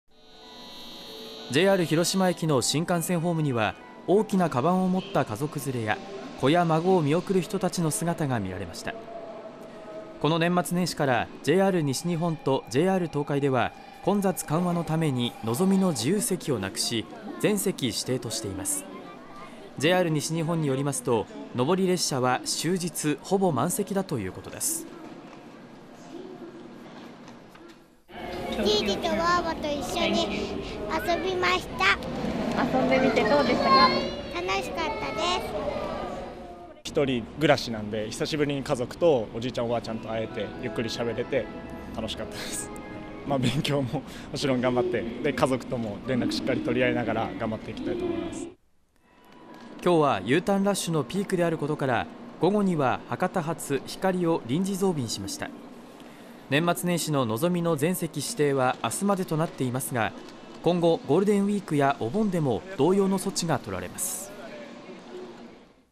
「のぞみ」全席指定の年末年始 JR広島駅でUターンラッシュピーク迎える 上り列車はほぼ満席
ＪＲ広島駅では、年末年始をふるさとで過ごした人たちのＵターンラッシュがピークを迎えました。（１月３日）